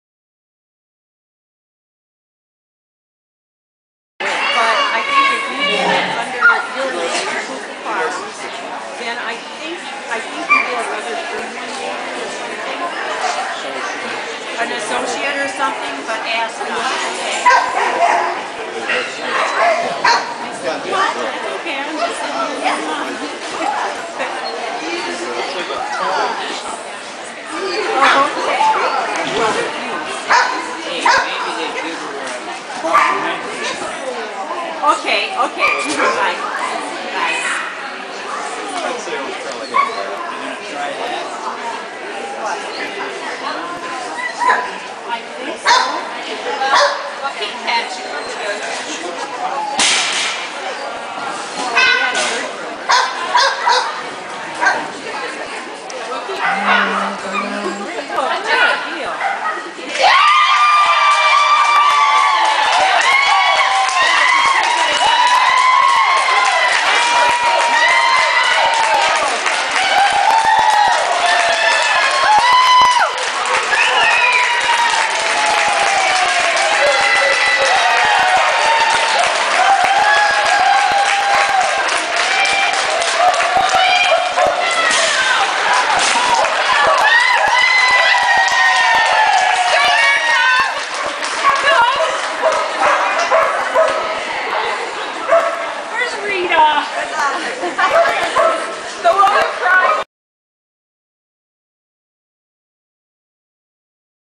MACH Schooner
CH MACH Schooner finished his MACH Dec 13,2008.  I added the thoughts that were going through my head!!!
Dexter Michigan under judge Cherie Treber
Ch MACH Reverie Smooth Sailing CD RE OF BSCA Agility Hall of Fame